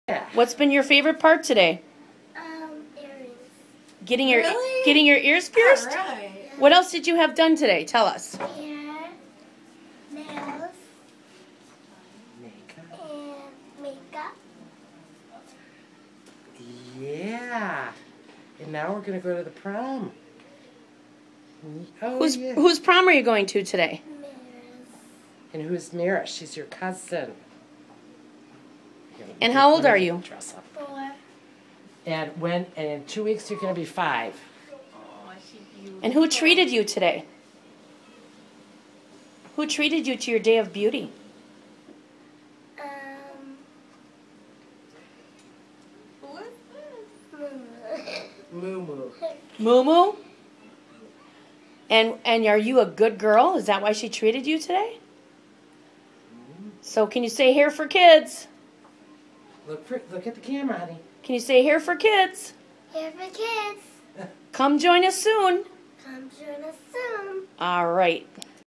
princess parties in Milwaukee, Waukesha, WI
Kids, children's dress up parties in Milwaukee, Waukesha, New Berlin, WI
We are turning off the lights, and turing on the black lights to make this party glow. Join us for an open party to make new friends. Glow hair, nails, make up, face paint, karaoke, dancing, limbo, and more!
kids manicures, pedicures, make up, dress up, fashion show, limbo, party venue, party planning for children
A Hair for Kids Salon LLC 14023 W. Greenfield Ave
New Berlin, WI